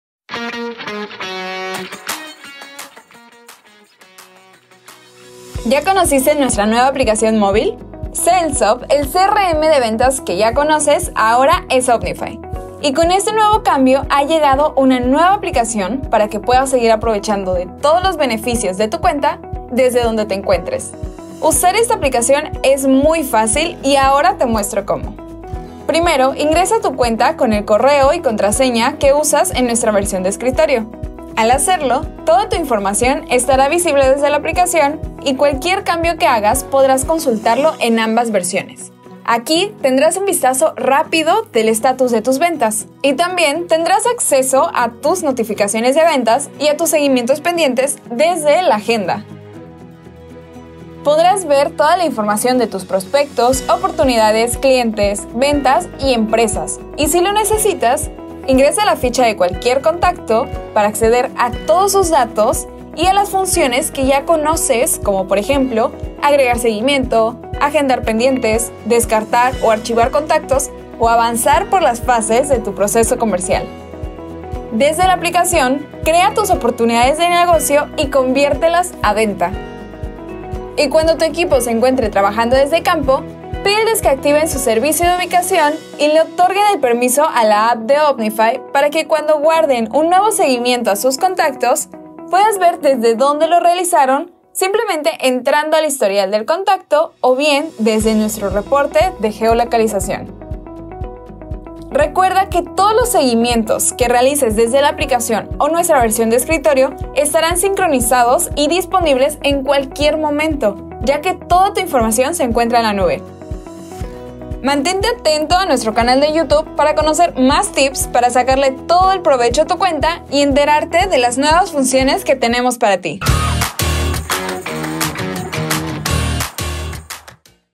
Conoce la NUEVA aplicación móvil que tenemos para ti
SalesUp! el CRM de ventas que ya conoces ahora es Upnify y con este cambio también ha llegado una nueva aplicación para que puedas seguir aprovechando todos los beneficios de tu cuenta desde donde te encuentres.

Usar esta aplicación es muy fácil, y ahora te muestro como...

📞Nuestros consultores te están esperando. Pide tu demo gratis